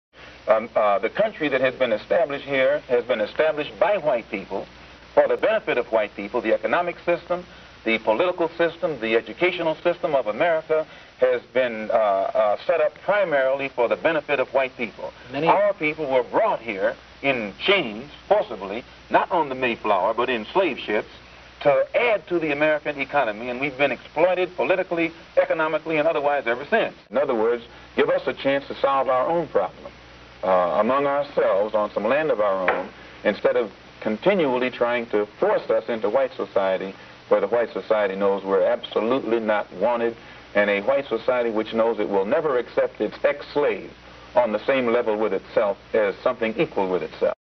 Solution to Black/White Race problems In America
How should black/white race problem be solved in America.
Should Blacks create their own country in America?